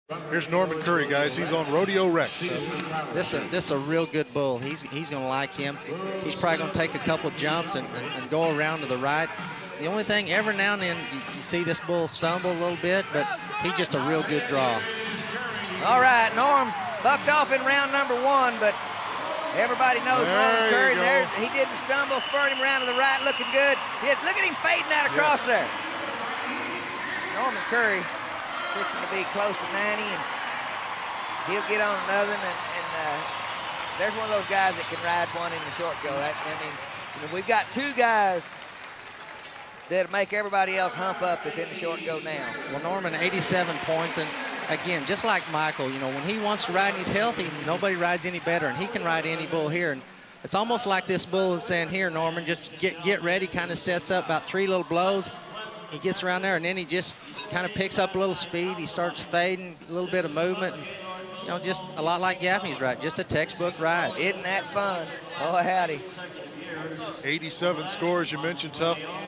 Norman Curry vs Rodeo Rex - 00 PBR Nashville (87 pts)
The 1990 NFR Average Champion "Stormin' Norman" Curry makes a nice ride on Bennie Beutler's C43 Rodeo Rex during Round 2 of 2000 Bullnanza-Nashville in Nashville, TN. Bull score: 42.00